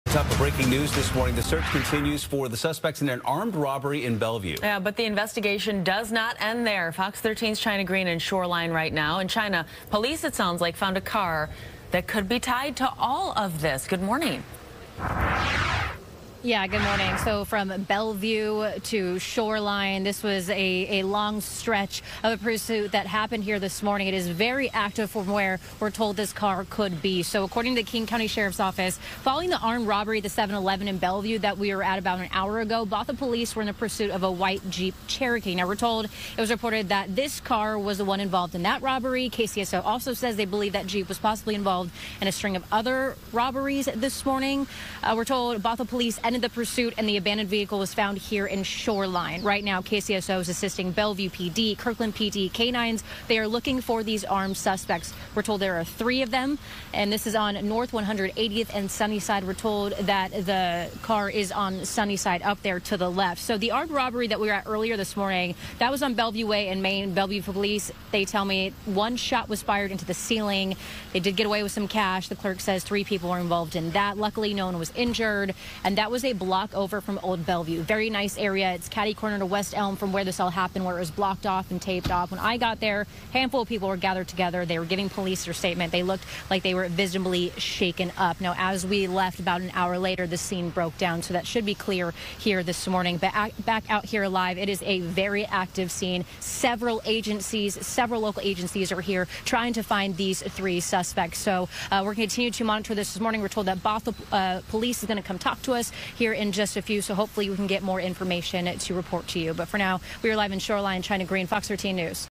Armed robbery suspects' vehicle abandoned in Shoreline
Police are continuing their search for suspects in an armed robbery in Bellevue, and they say they found a vehicle that may be connected to a string of robberies. It was abandoned in Shoreline.